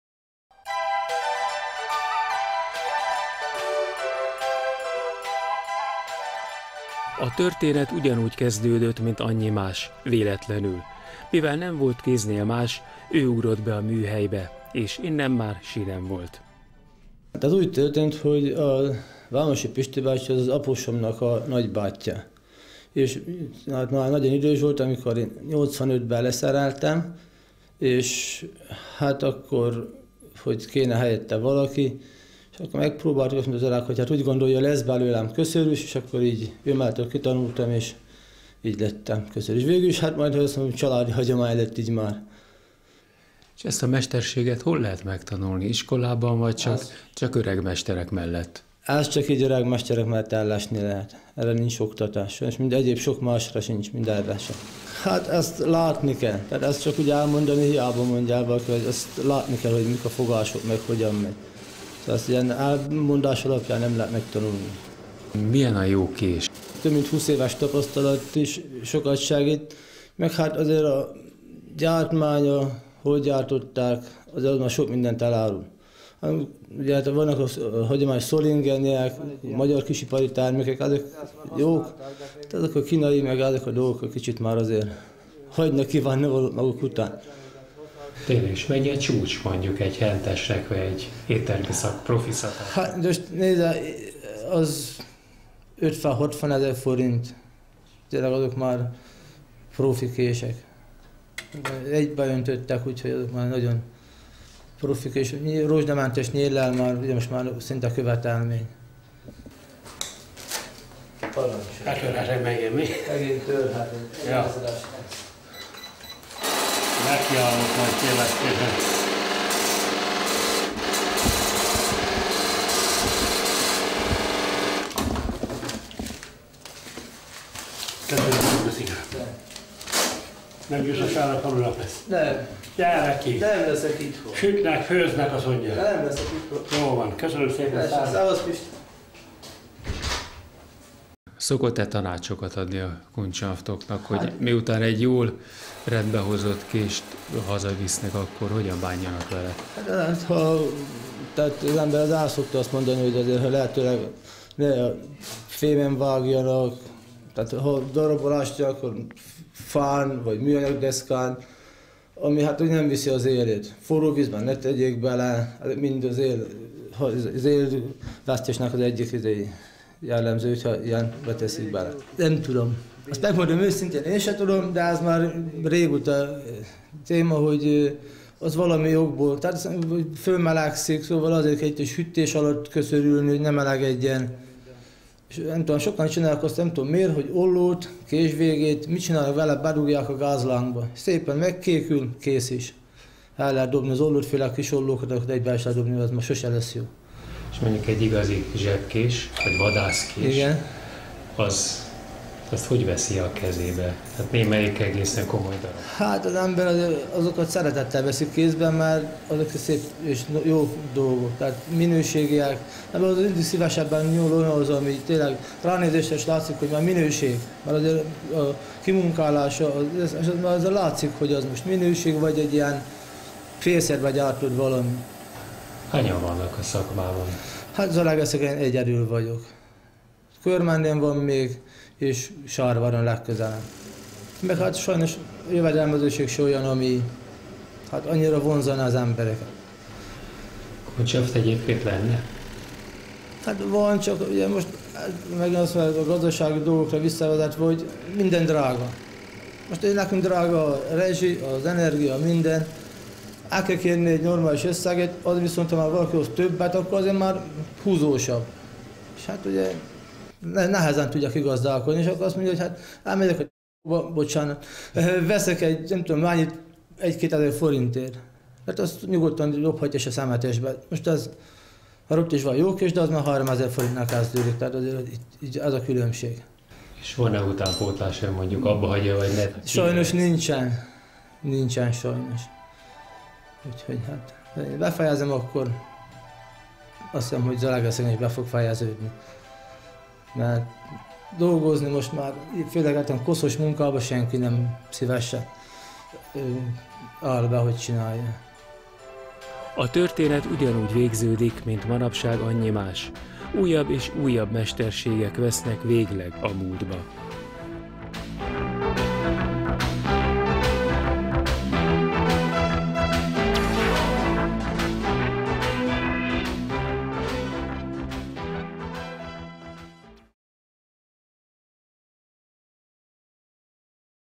Sebestyén Gyula műköszörűs
Az utolsó műköszörűs Zalaegerszegen/miniportré/